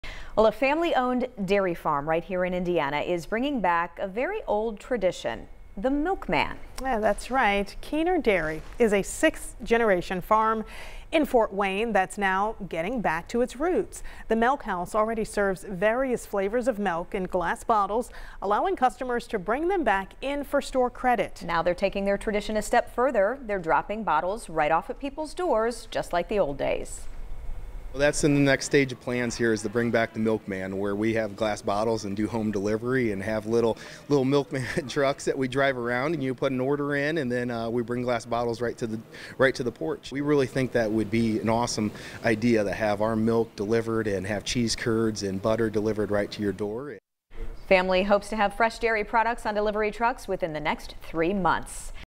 Indiana dairy farm bringing back the 'Milkman'
Kuenert Dairy in Fort Wayne family says they hope to have fresh dairy products on delivery trucks within the next three months.